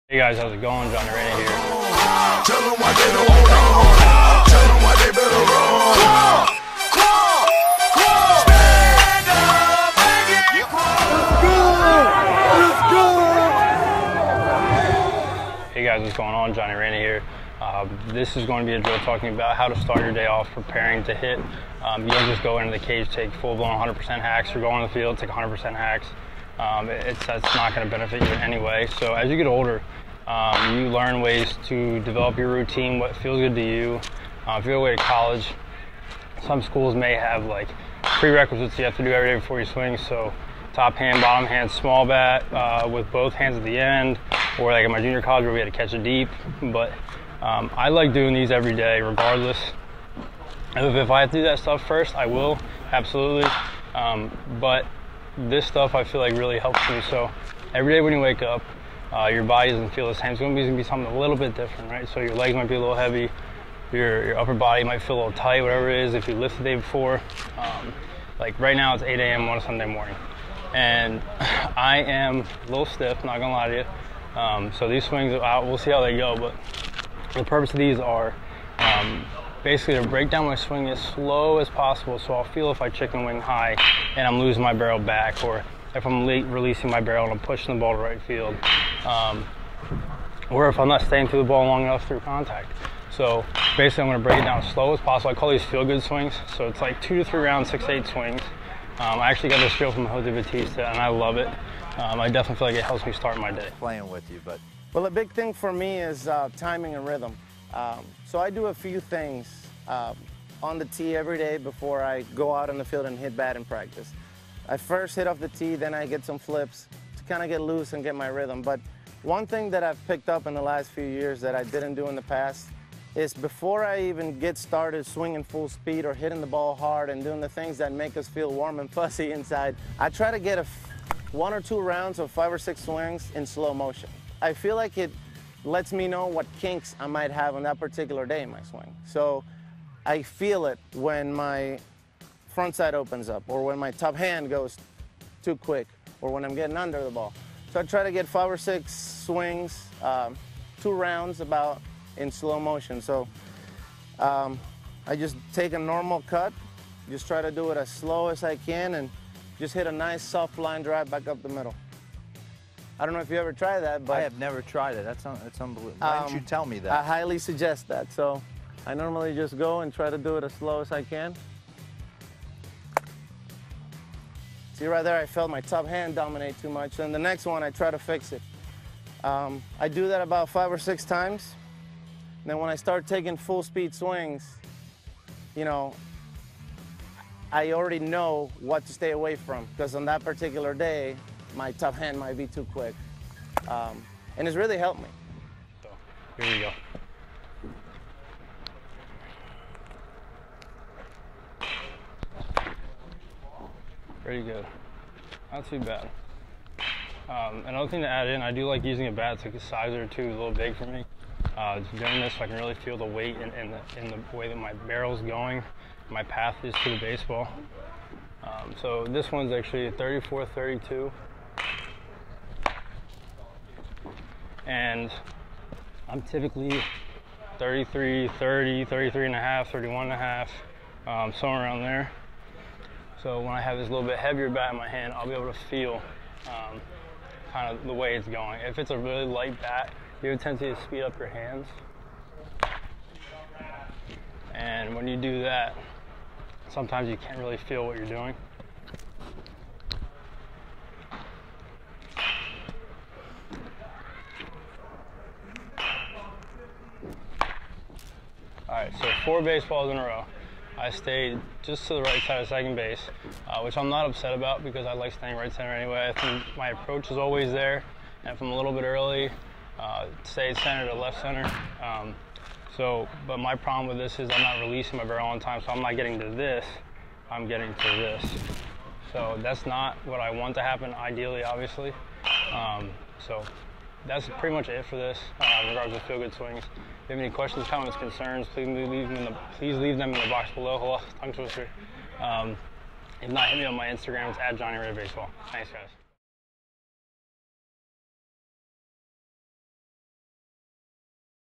Hitting Drills - Feel Good Swings
Picked this drill up from a Jose Bautista Diamond Demo Back in 2015... (during the peak years of his career when he was a MONSTER) enjoy !

The Eyes of Baseball:

The Secret Arts Within the Game: